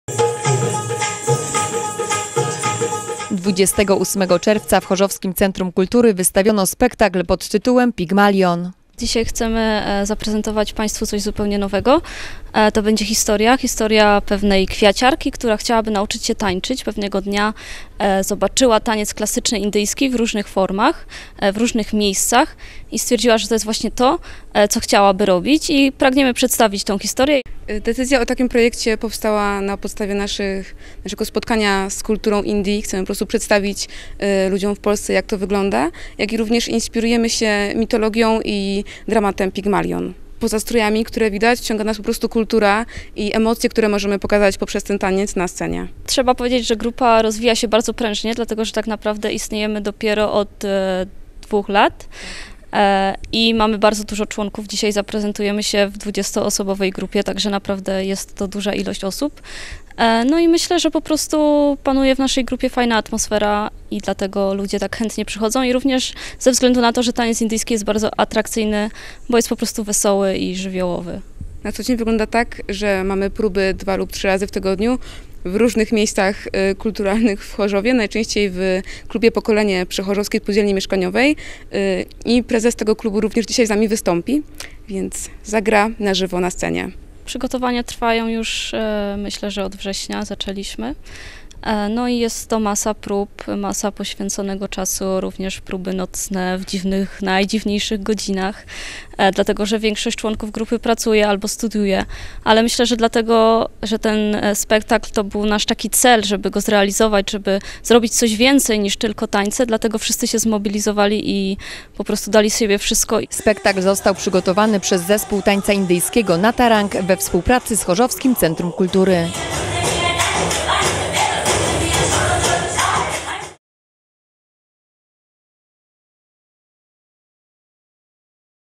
SPEKTAKL TANECZNY "PIGMALION"
28 czerwca w Chorzowskim Centrum Kultury wystawiono spektakl pod tytułem "Pigmalion".